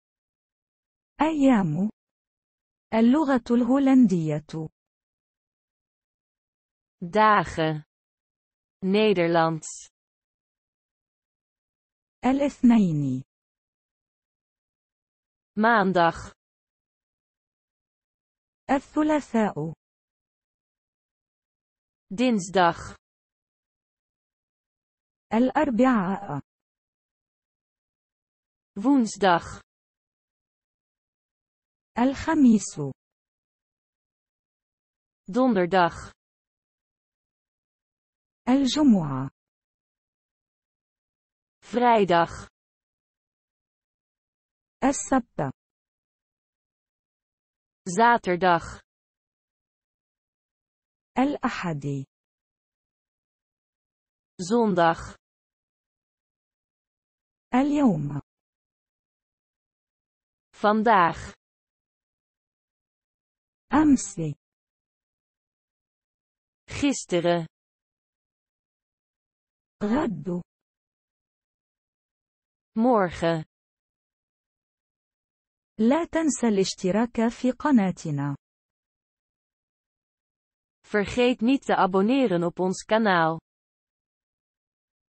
أيام (اللغة الهولندية) (ar-nl)
Fone TV
ar-nl-23
Dagen (Nederlands)
الاثنين - Maandag
الثلاثاء - Dinsdag
الأربعاء - Woensdag
الخميس - Donderdag
الجمعة - Vrijdag
السبت - Zaterdag
الأحد - Zondag
اليوم - Vandaag
أمس - Gisteren
غد - Morgen